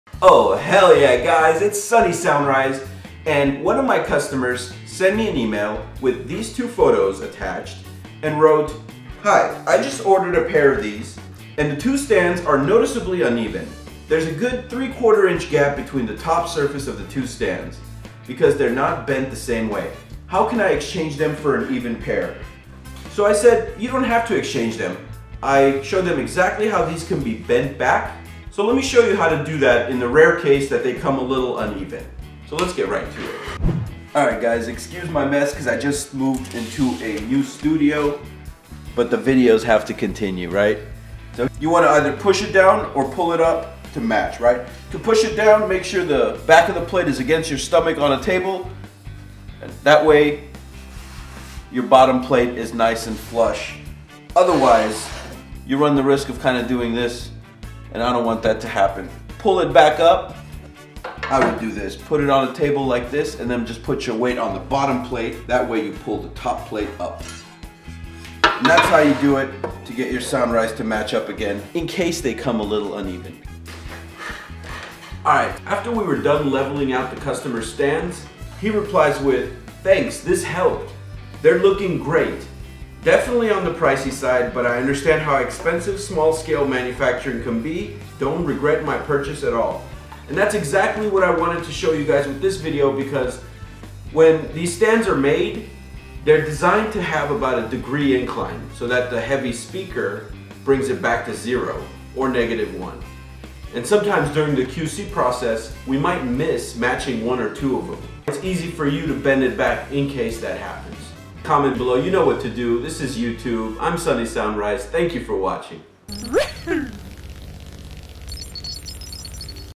Fixing Uneven Soundrise Stands
If your Soundrise speaker stands happen to arrive with an offset top plate, don't worry. You can simply push or pull to bring them back to even.